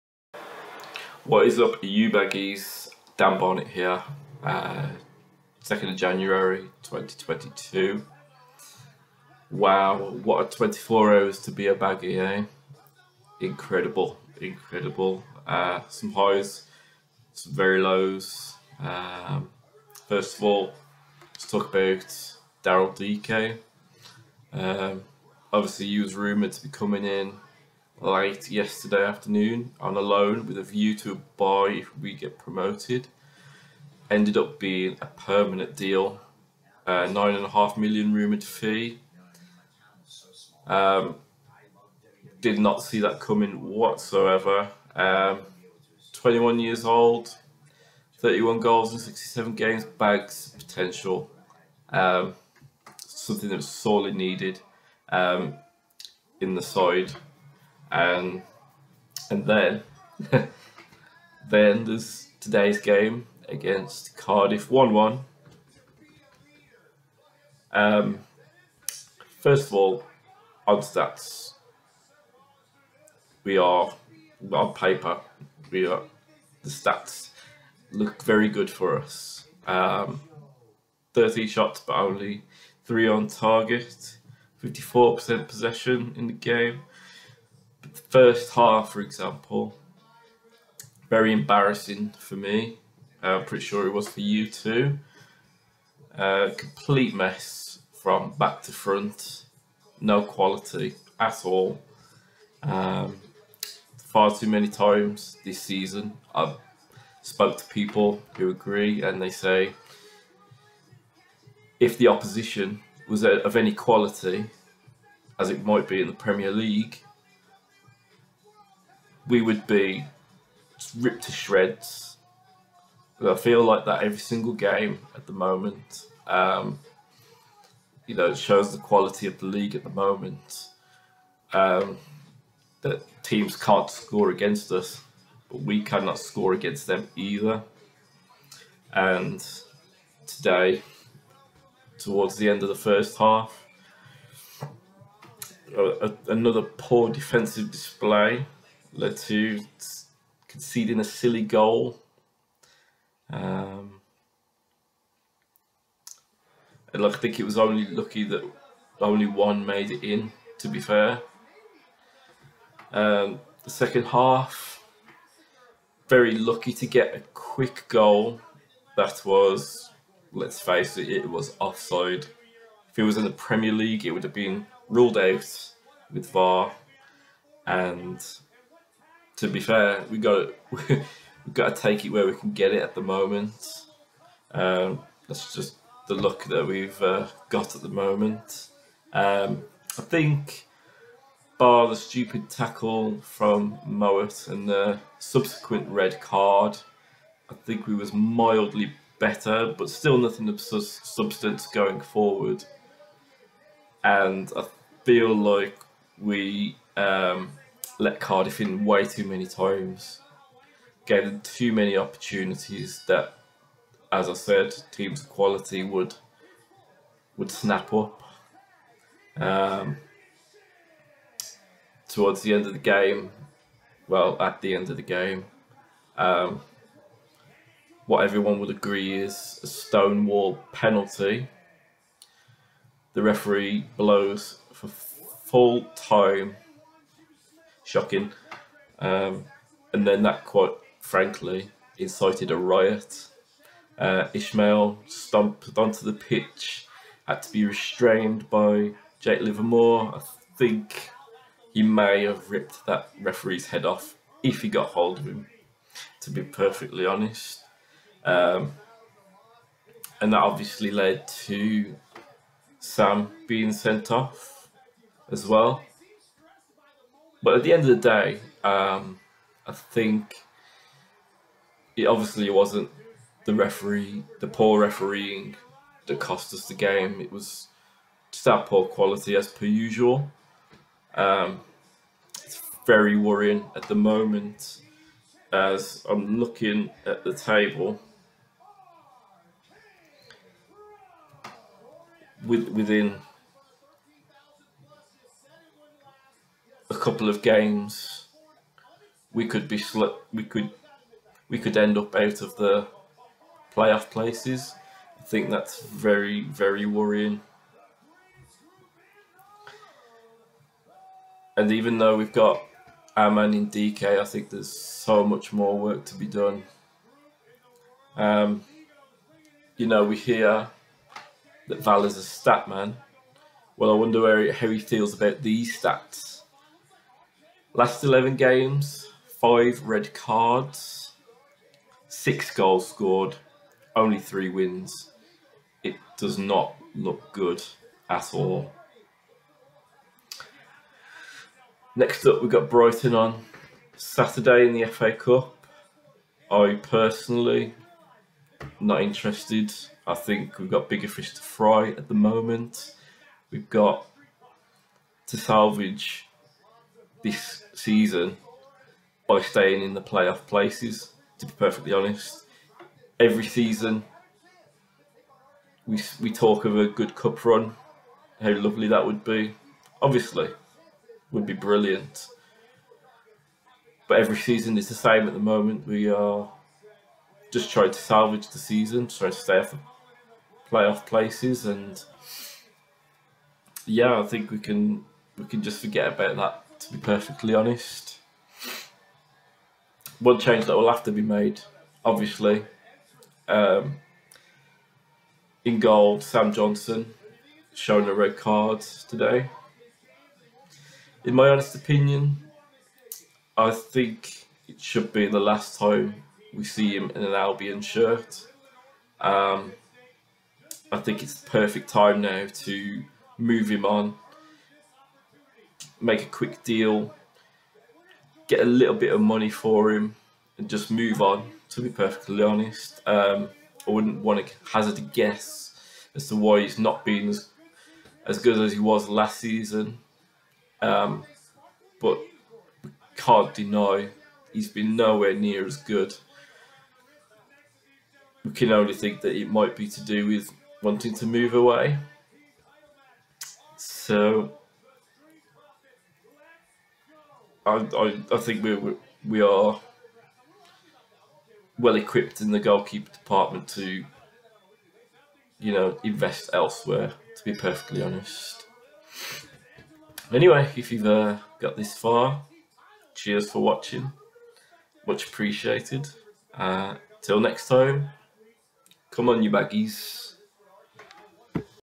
Daryl Dike arrives at West Bromwich Albion! WBA 1 - Cardiff 1 Mayhem! Johnstone and Mowatt see RED!!
Talk about Daryl Dike arriving at the Hawthorns! Also about today's 1-1 draw with Cardiff and the on-field chaos that followed!